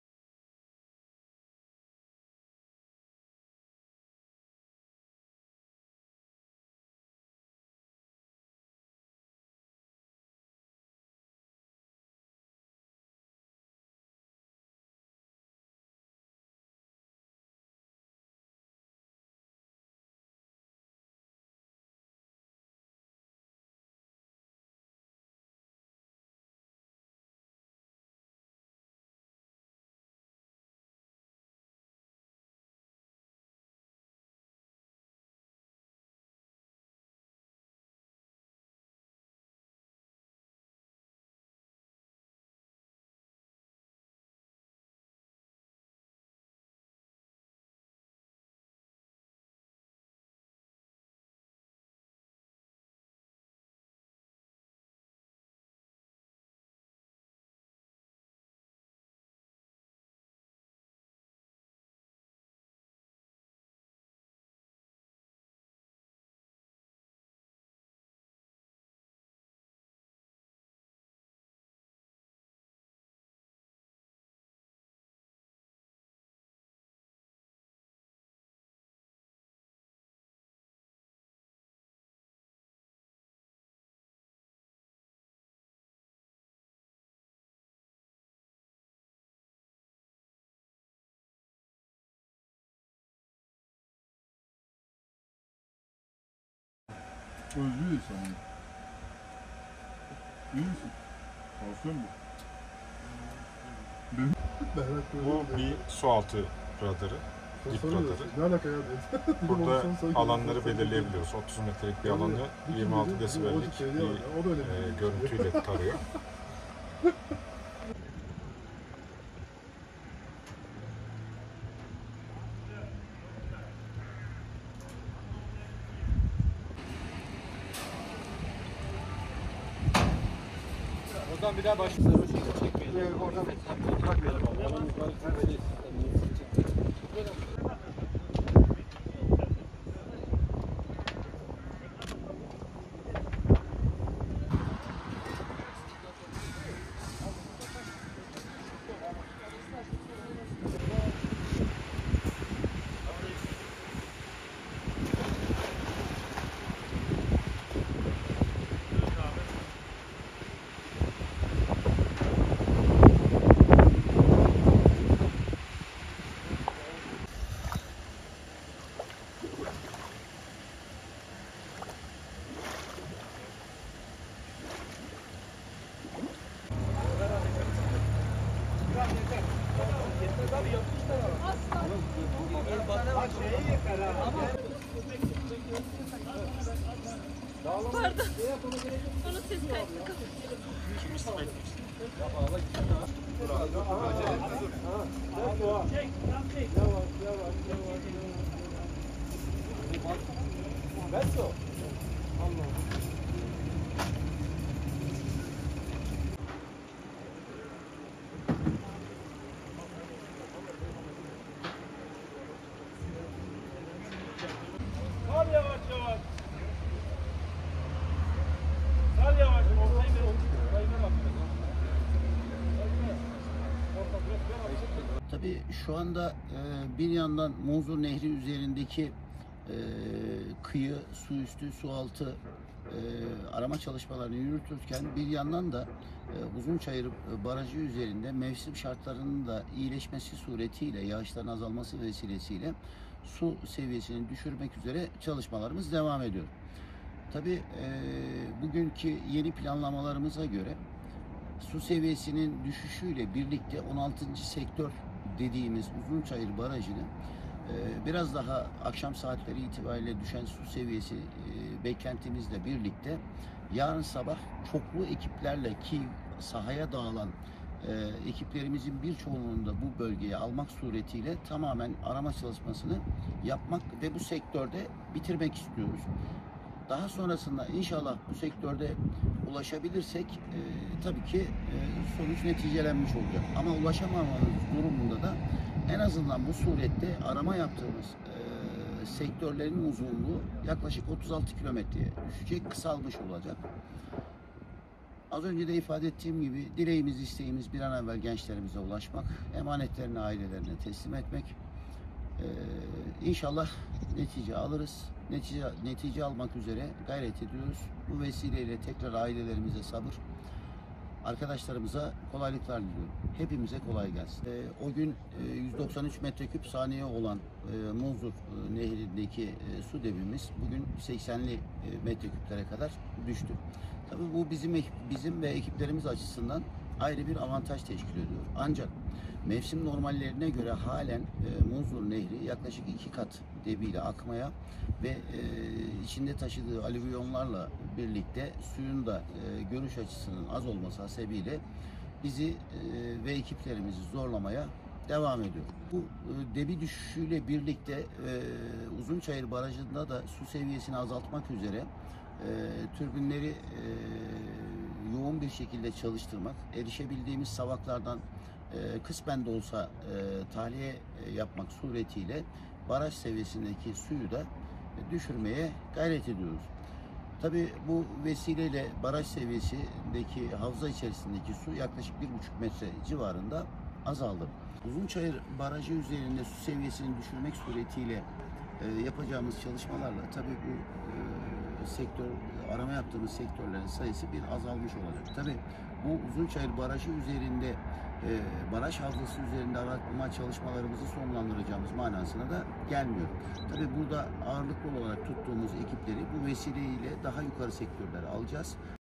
Kayıp 3 Genç için 10 Milyon Metreküp Su Boşaltıldı, Debi Yarıya Düştü
Tunceli'de Munzur Çayı'na uçan otomobildeki kayıp 3 genç için arama çalışmaları kapsamında kapakları açılan Uzunçayır Baraj Gölü'nde 10 milyon metreküp su boşaltıldı. Gençlerin suya düştüğü gün 193 metreküp olan Munzur Çayı'nın debisi 80 metreküpe düştü. Köprü ayaklarına çelik halatların yerleştirildiği arama çalışmalarının 15'inci gününde gençlere ulaşılamadı.
Serhat Ozan YILDIRIM/ TUNCELİ, (DHA)-